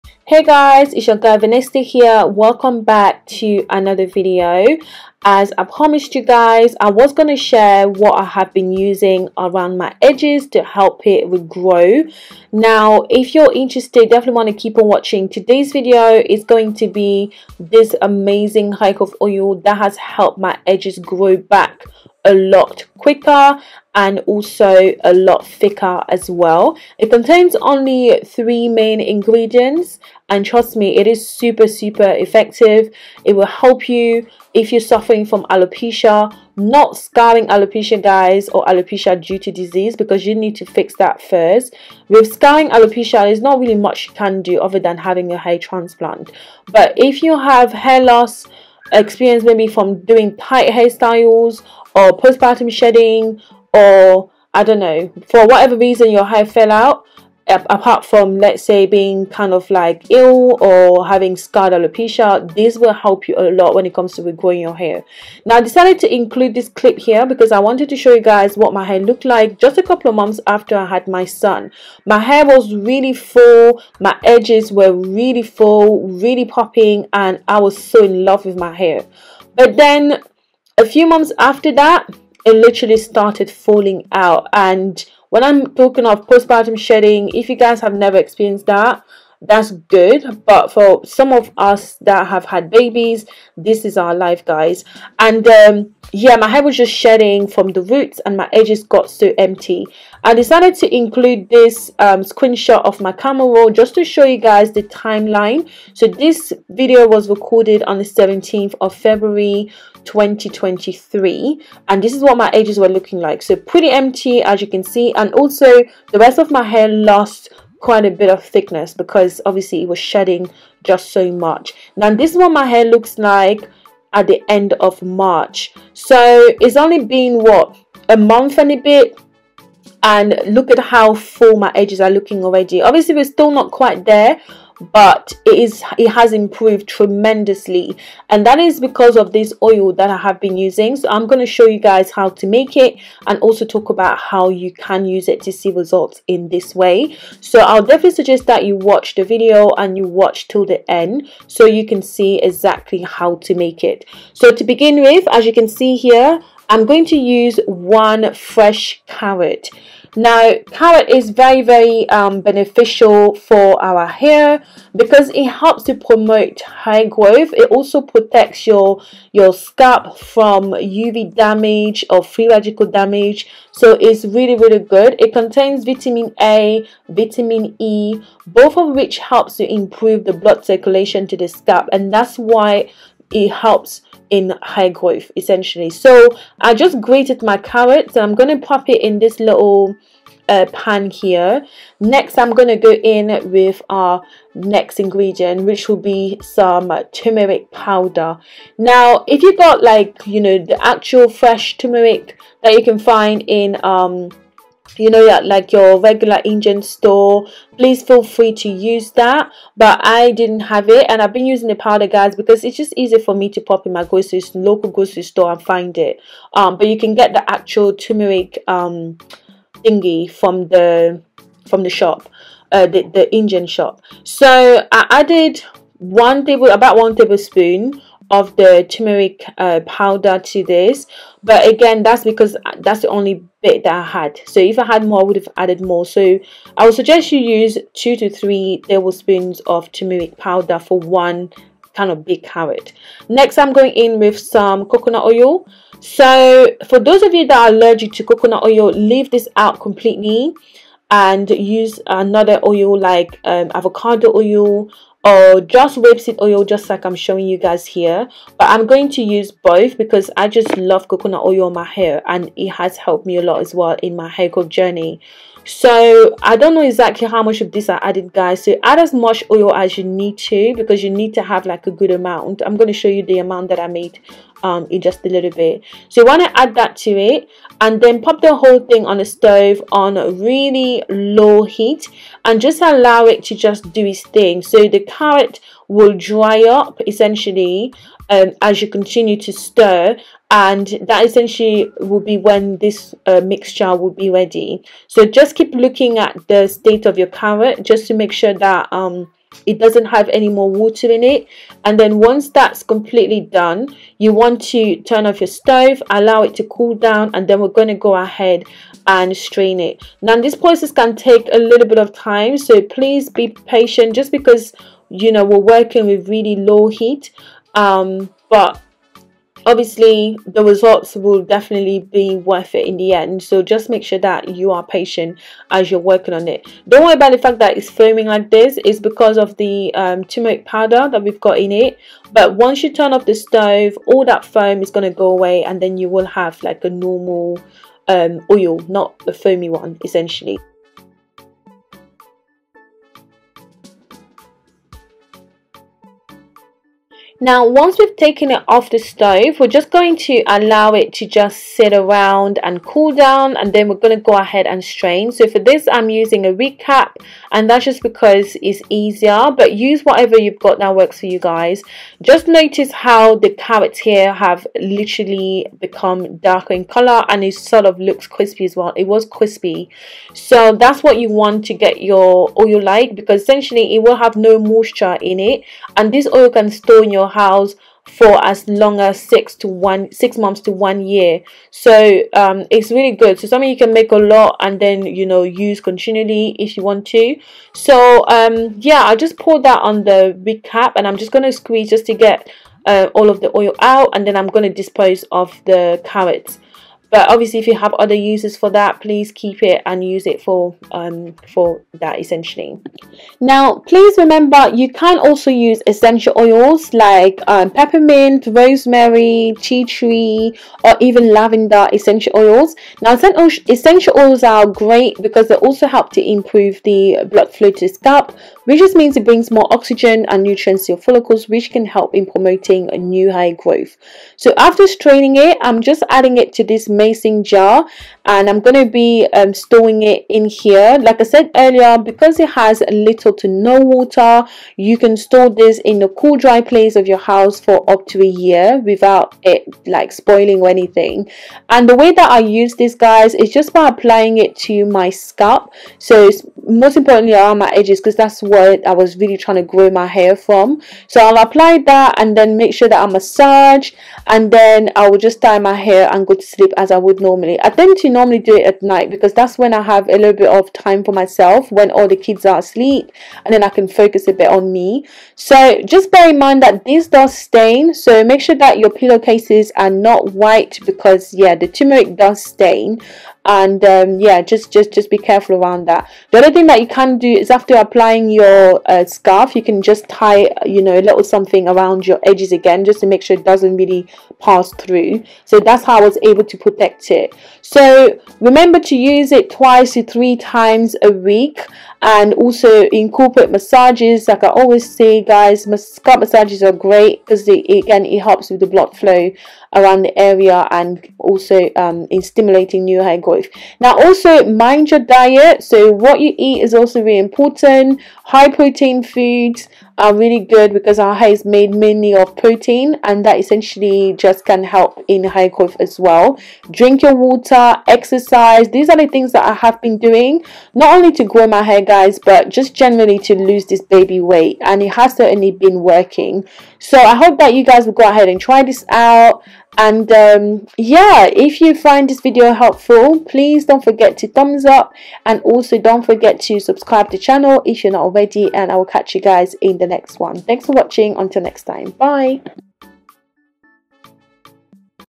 😱Way Better Than Cloves,This Grows Hair Like CRAZY! Your Hair Will Go Crazy From 1st Use!🔥🔥
hey guys, this is the video you've all been waiting for! I share with you all how I grew my edges back in 4 weeks with this turmeric hair growth oil. My Edges Grew Back In 4 weeks I could not Believe my Eyes! This Grows Hair Like CRAZY! 🔥🔥🔥this is way more effective than cloves and you be shocked at the results. contains only 3 main ingredients!

0:00-0:38-INTRO
0:38-2:21-PROOF
2:21-07:23- TUTORIAL
7:23-9:06- APPLICATION

Ingredients used:

1 carrot
turmeric powder
coconut oil(optional)
rapeseed oil